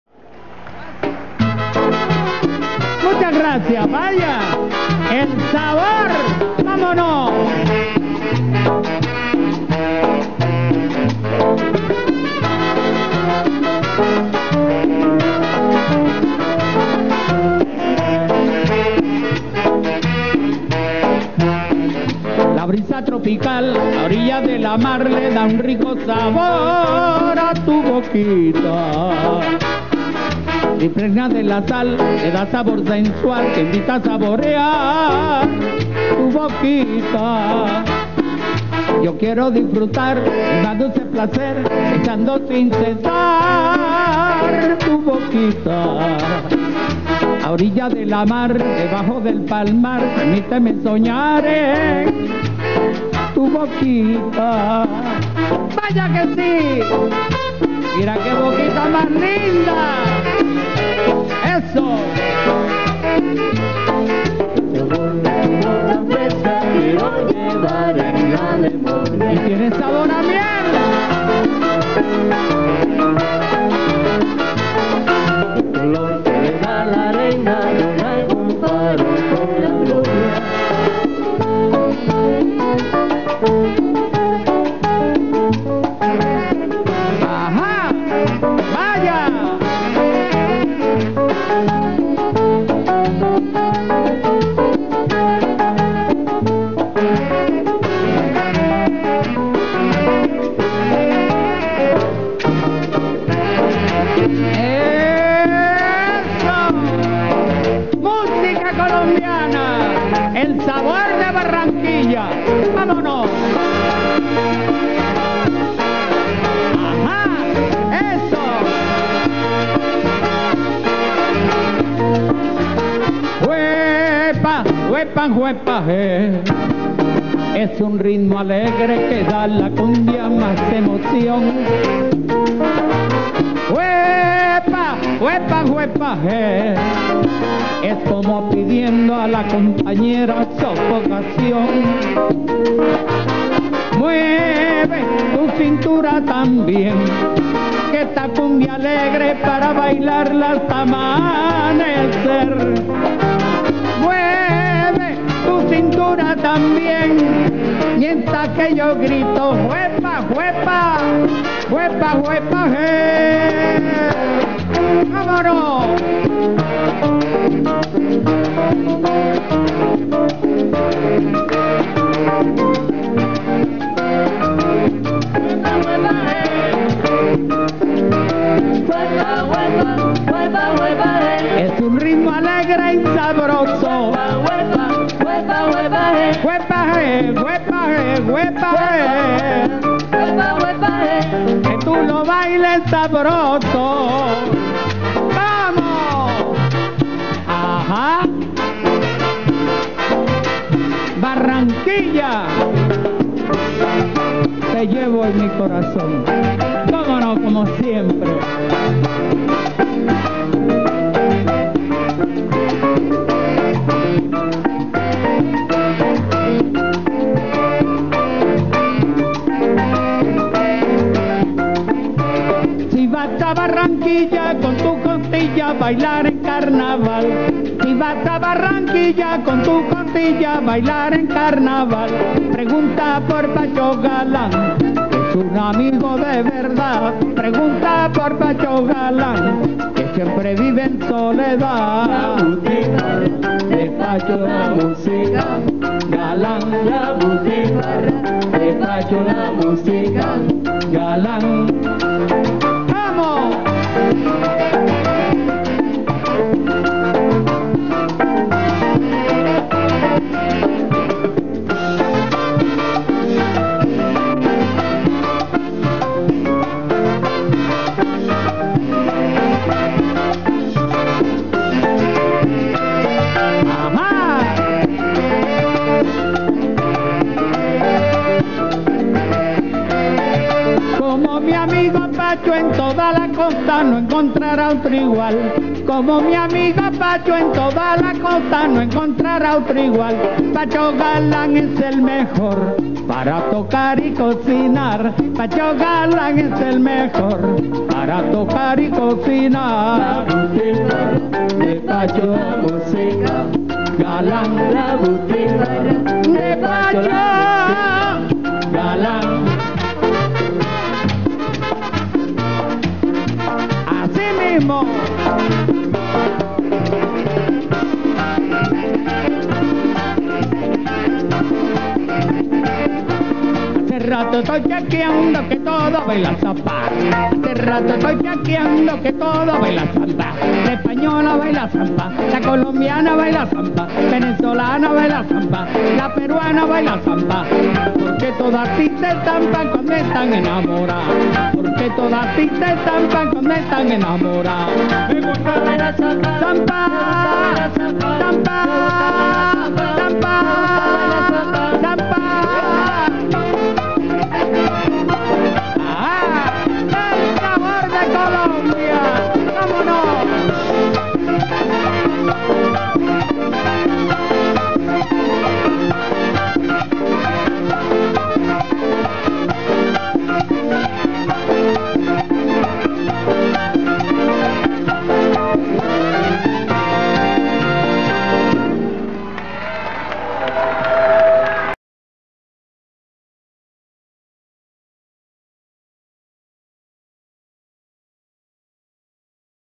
CHEO GARCIA CANTA SUS EXITOS CON LA ORQUESTA LA PLAYA DE COLOMBIA
COLECCION DE ORO DE NELSON ZABALA (NELBILLO)